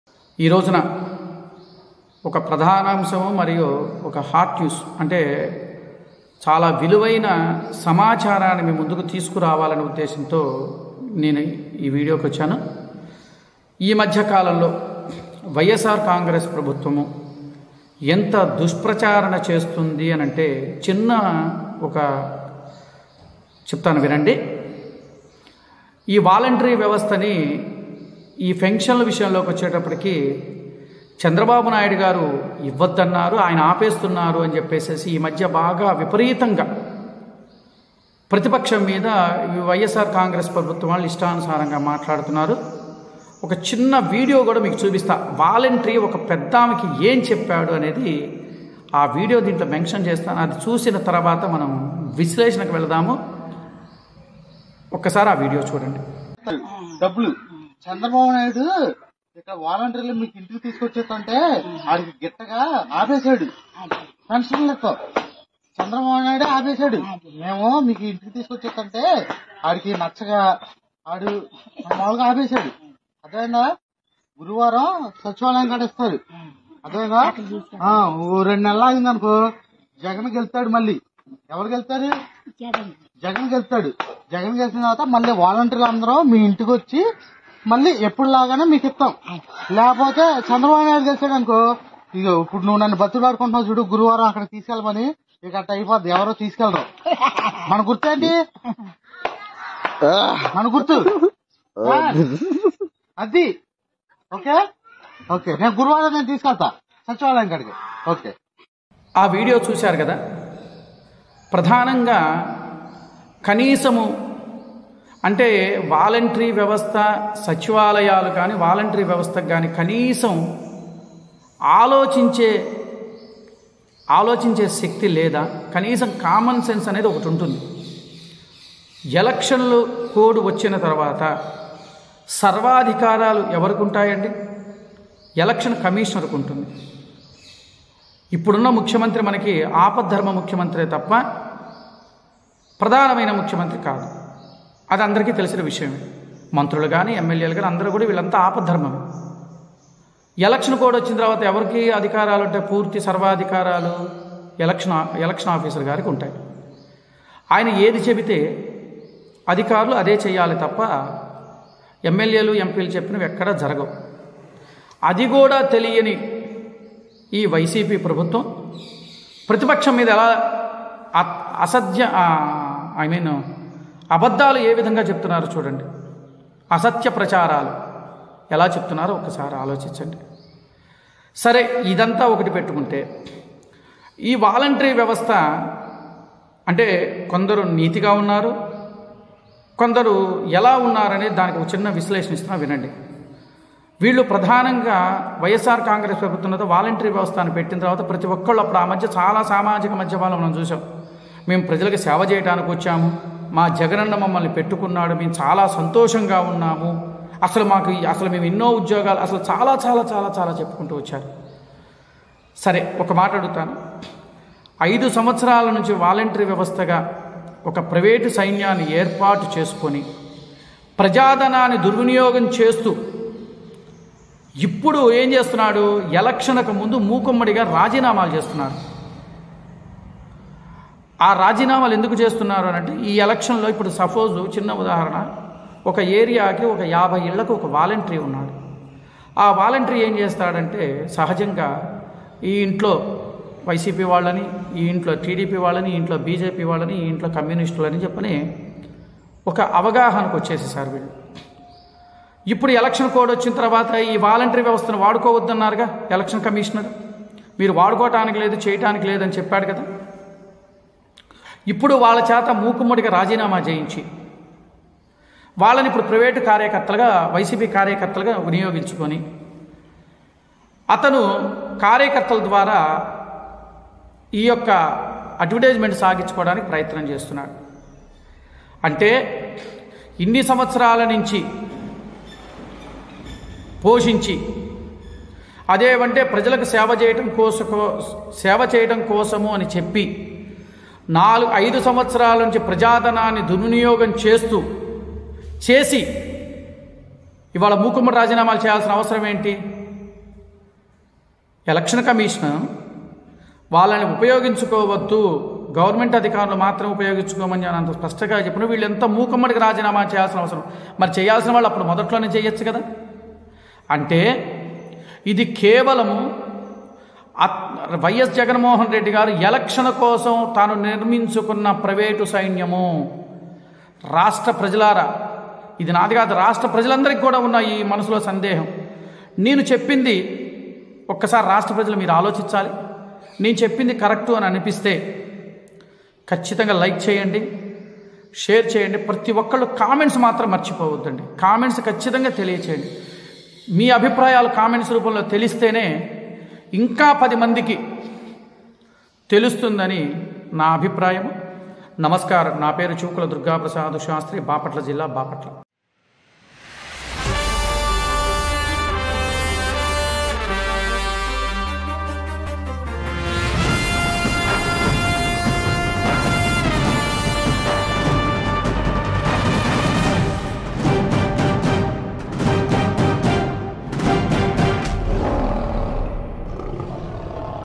వైసీపీ ని దుమ్మురేపిన పూజారి🔥 AP Poojari Reaction AP Volunteers issue | Ys Jagan | Chandrababu
Leo Politics

Leo Health

Leo Entertainment

Leo Devotional